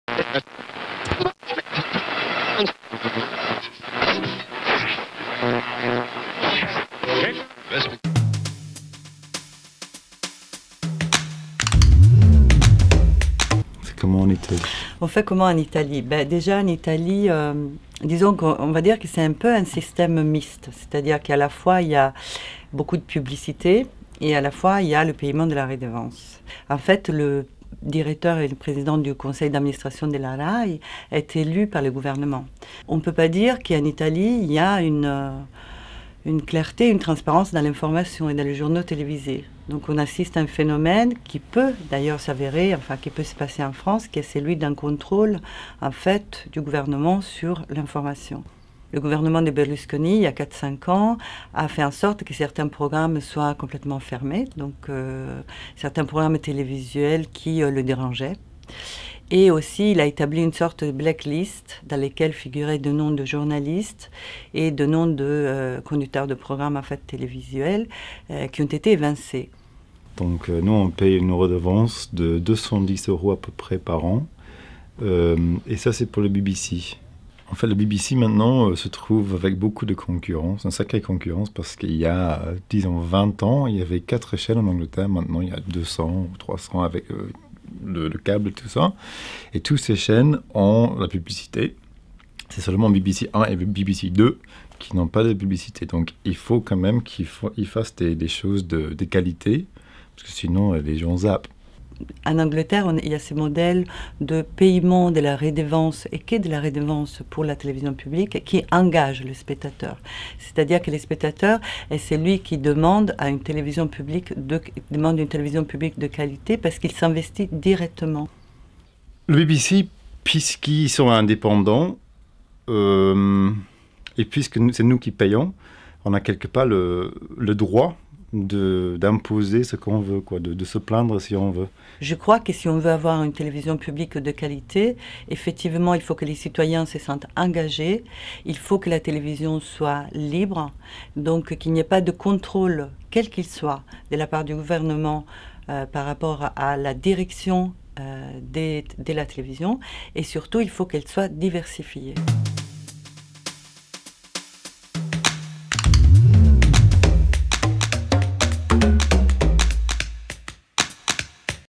Télévisons
Licia Eminenti et Jon Hutchinson télévisent sur : La BBC et la RAI